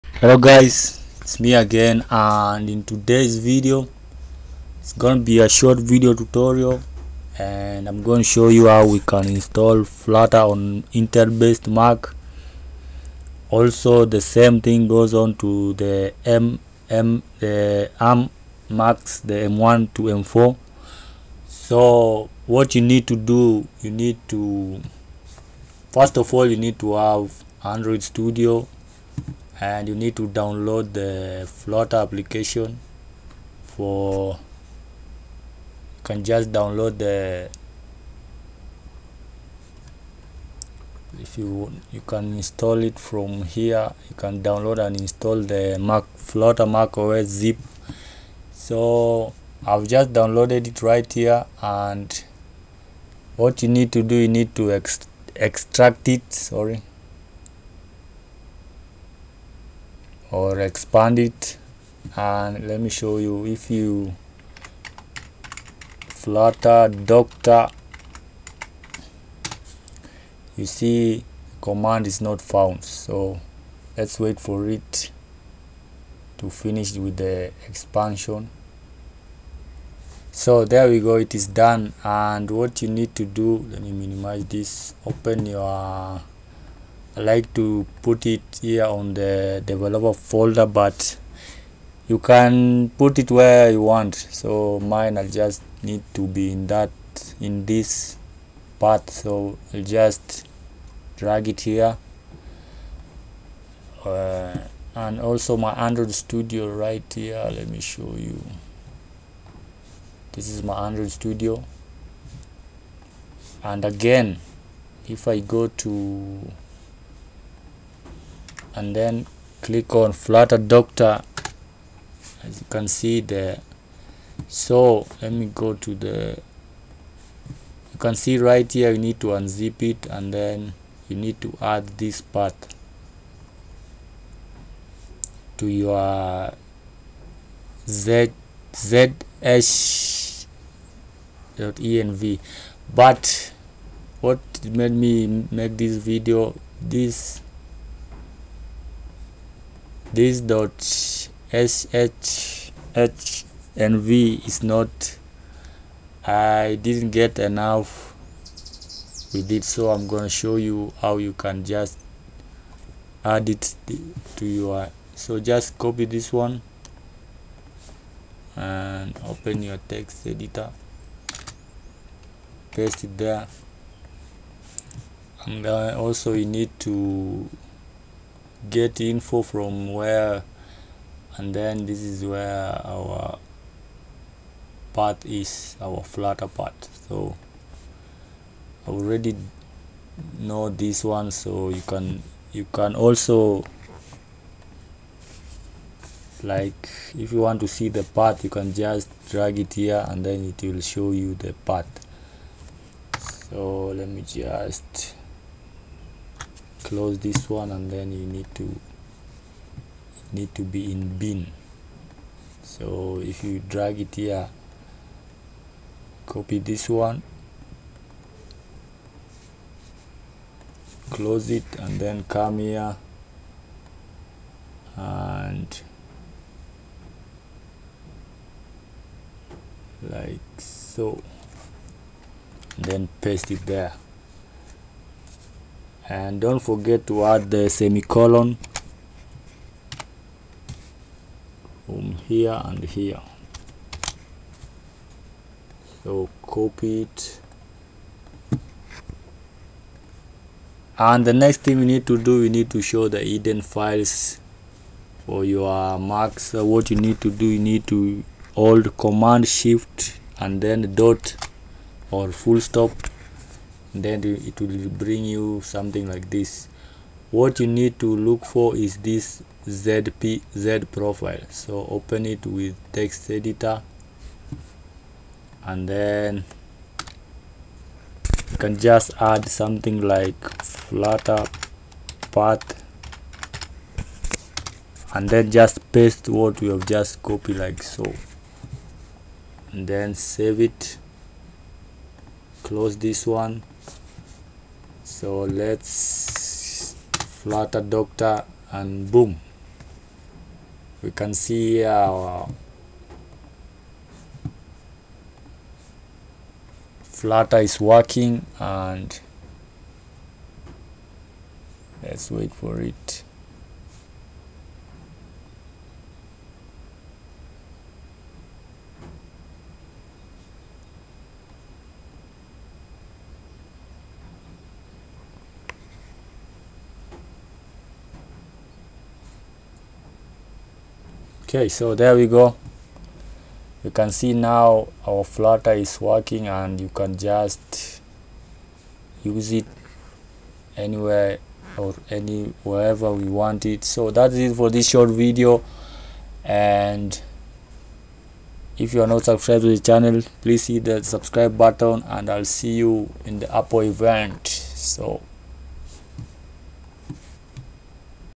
Install Flutter on mac intel/M1-M4
Short video how to install flutter in mac